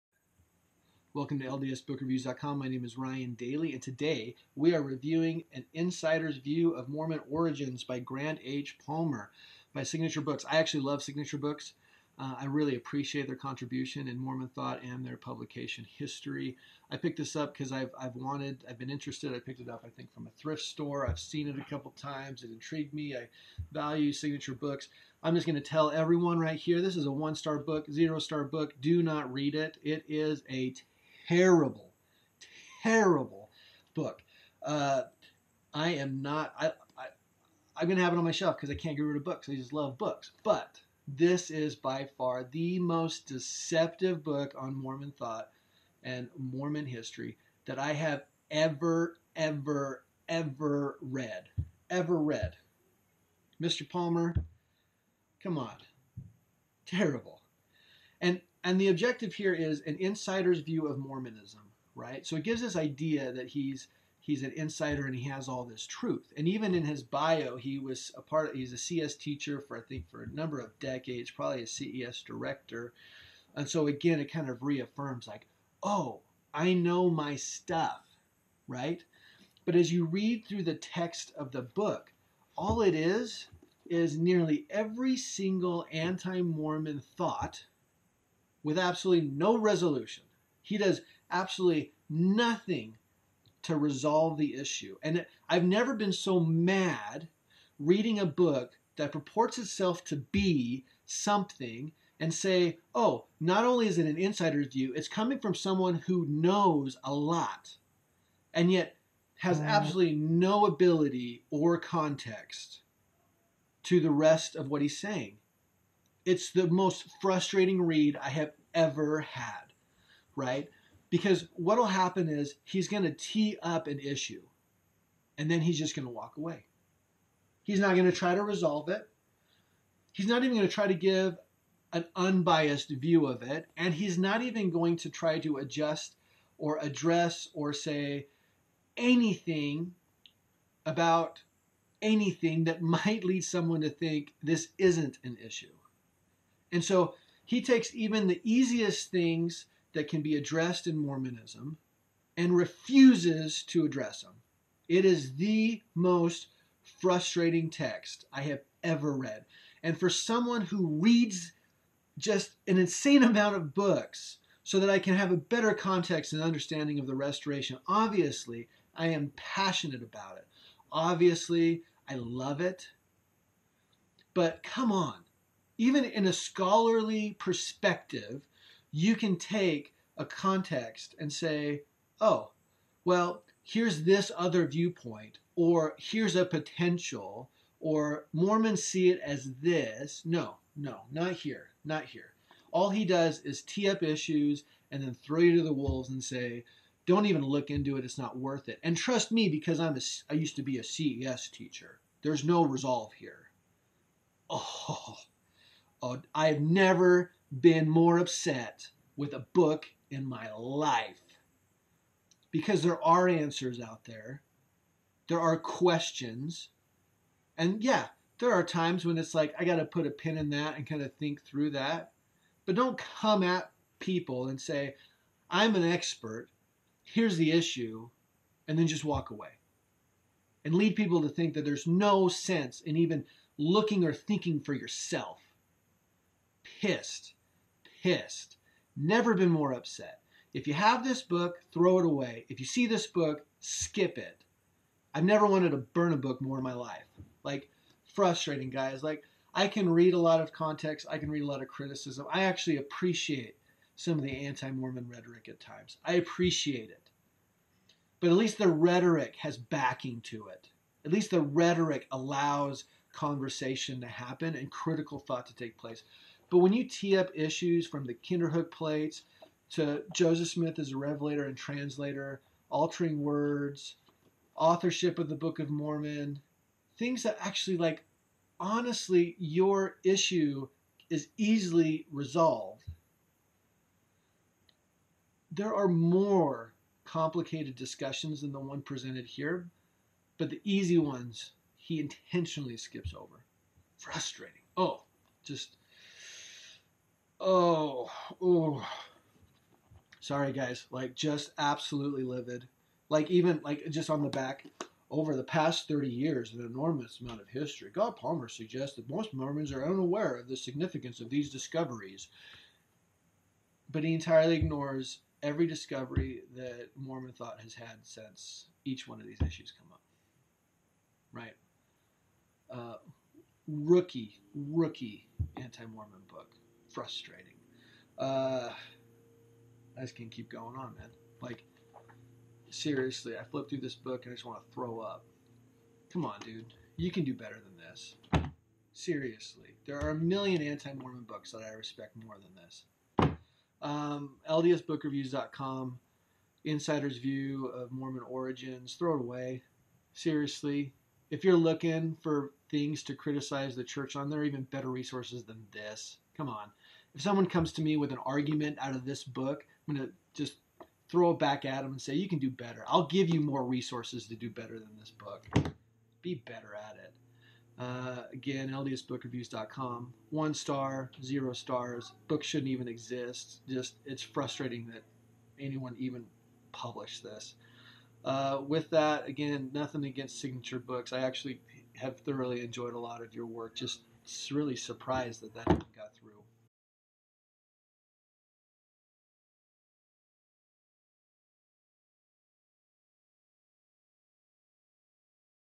An Insider's View of Mormon Origins - Grant H. Palmer (Review)
This is a summary of a review provided by LDSBookReviews.com on An Insider's View of Mormon Origins by Grant H. Palmer.  For more reviews please follow this channel, or visit LDSBookReviews.com - or click on this direct